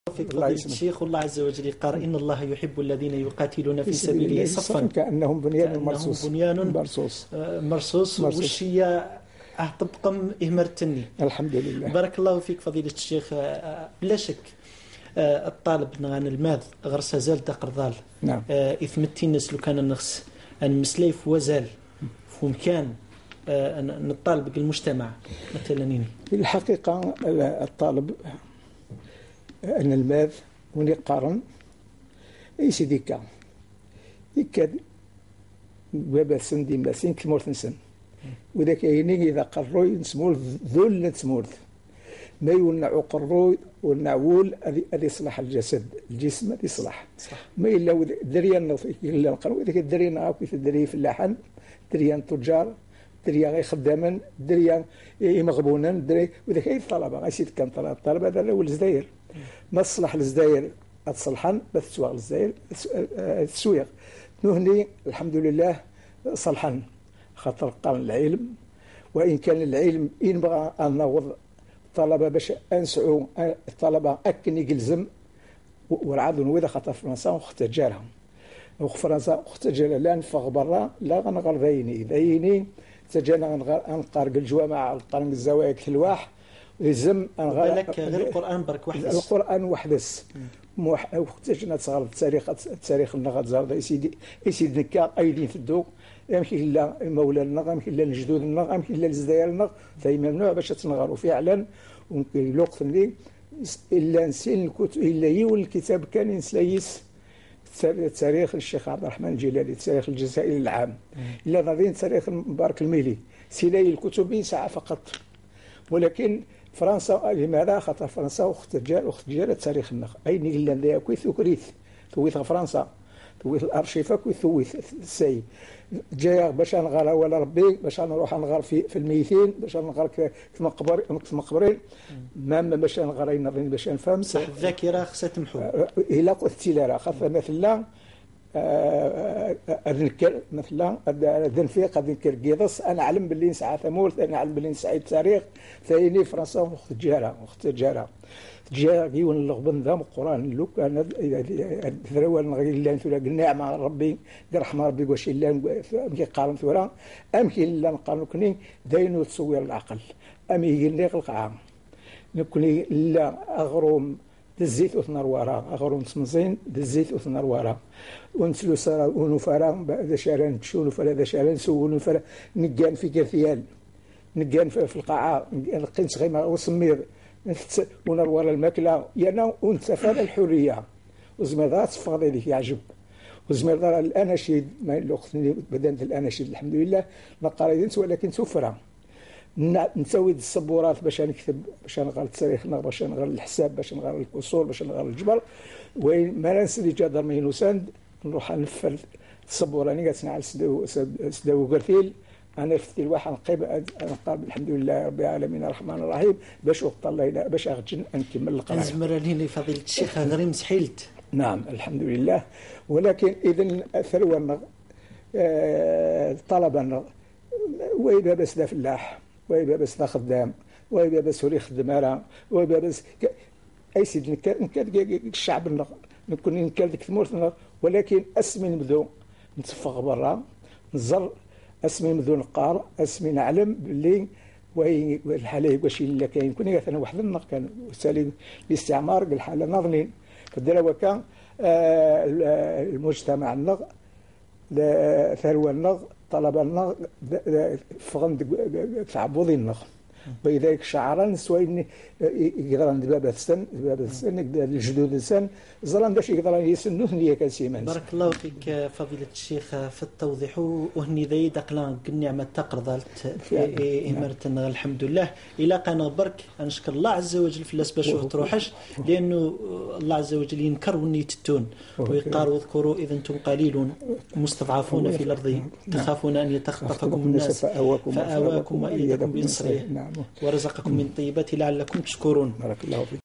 TV Thamazighth et ses Programmes destinés aux Kabyles !!!!! 2
On se régale bcp vraiment, merci notre chère TV Thamazighth, un programme riche et innovateur, ça c'est du Kabyle, de l'histoire, de traditions, des valeurs et des coutumes de chez  vous, Merci encore  ( à bon entendeurs )